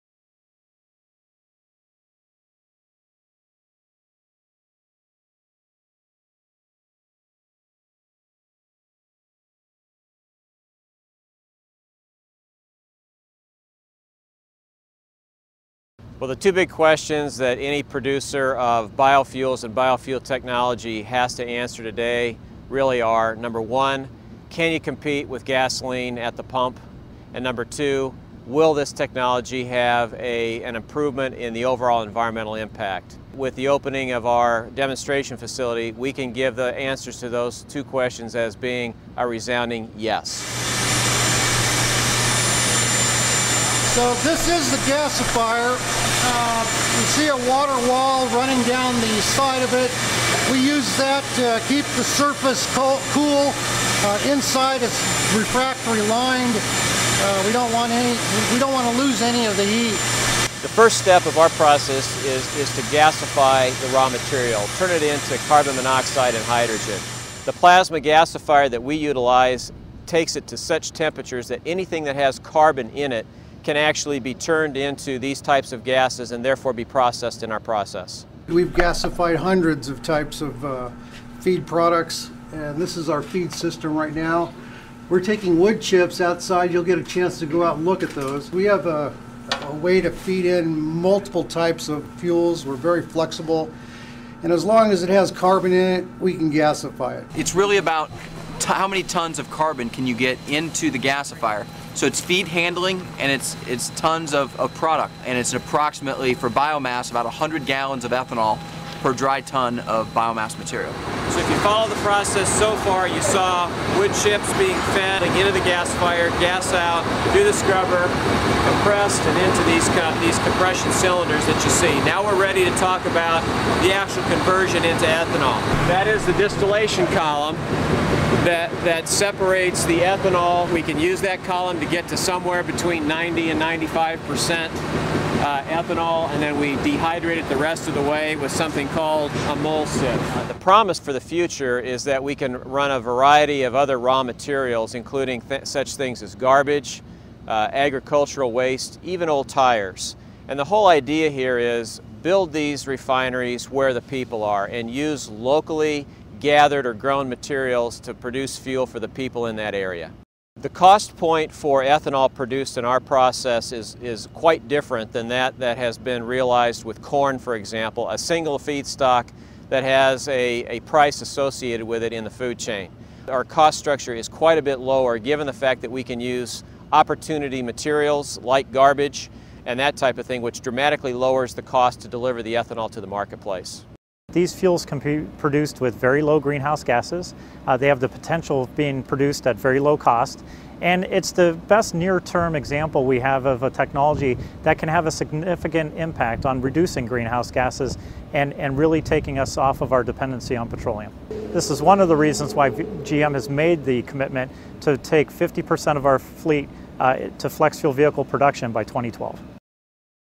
Inside Coskata's Flexible-Feedstock Ethanol Facility
A tour of Coskata's semi-commercial facility in Madison, PA, where ethanol is produced from a wide variety of feedstocks using plasma gasification and specially engineered microorganisms.